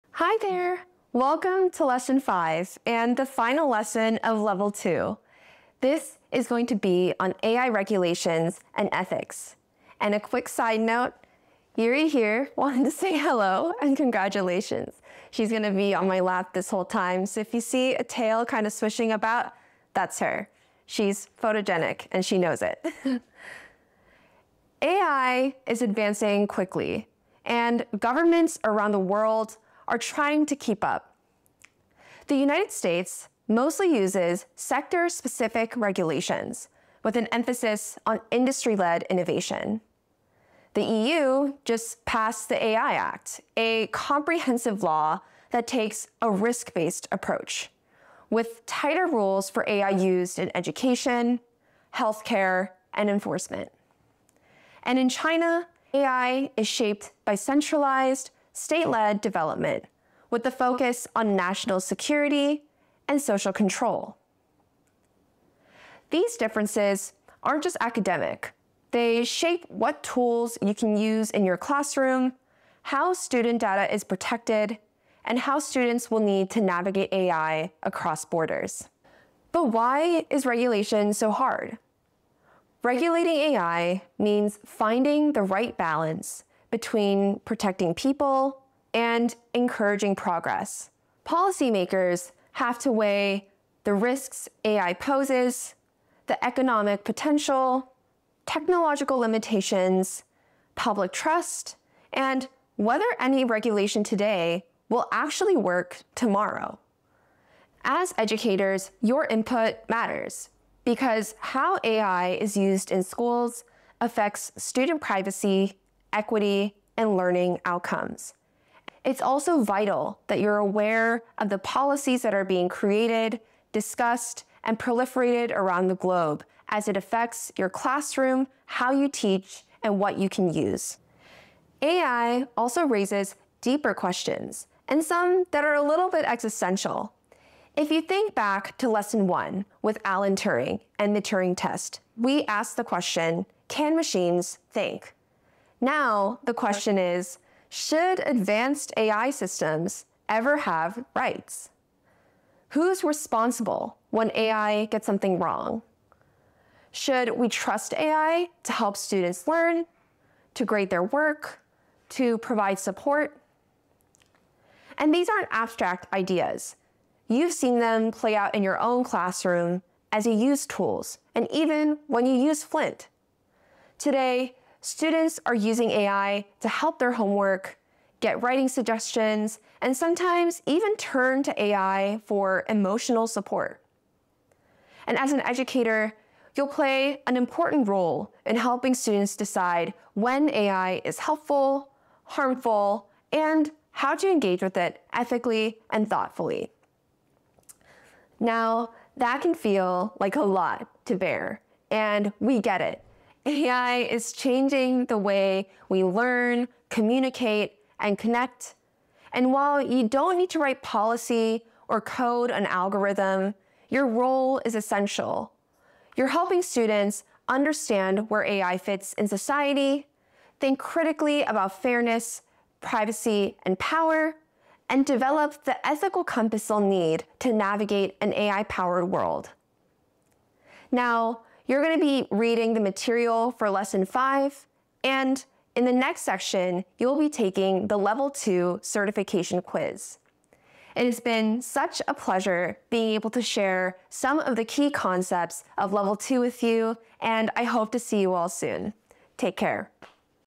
Level 2 Lesson 5: AI Regulations, Policies, and Ethics — Flint AI Literacy Course for Teachers
What rules govern the use of AI and how should we think about its role in society and education?

In this lesson, educators explore the evolving landscape of global AI regulation, including how the U.S., European Union, and China take different approaches to balancing innovation, safety, and control. We also examine the ethical questions surrounding AI, such as responsibility, transparency, and the rights of intelligent systems.

From data protection laws to philosophical debates about AI’s impact on learning, this lesson helps teachers understand the regulatory and moral frameworks that influence which tools can be used in schools and how to use them responsibly.

Chapters:
00:00 Introduction
00:24 Global policy approaches
01:19 Difficulty in regulating AI
02:10 Ethical questions with AI
02:53 AI use by students

Explore more of our resources here: flintk12.com

Join our webinars to see how AI can be used in the classroom